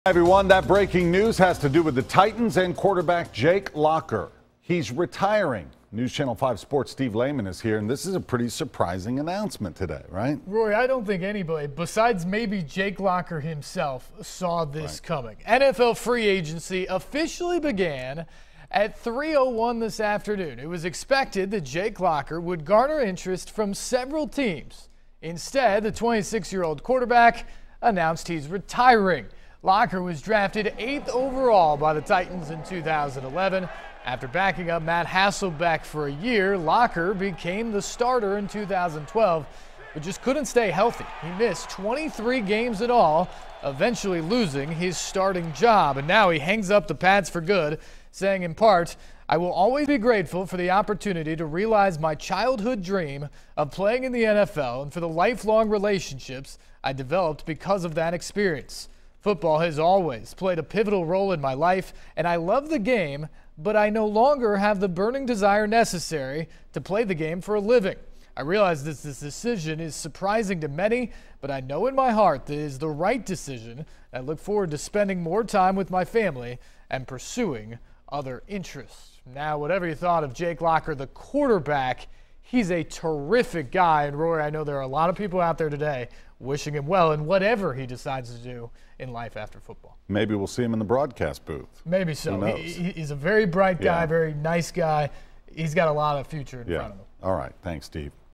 Titans' QB Jake Locker Announces Retirement
Jake Locker, a former starting quarterback for the Tennessee Titans, has announced his retirement from the National Football League.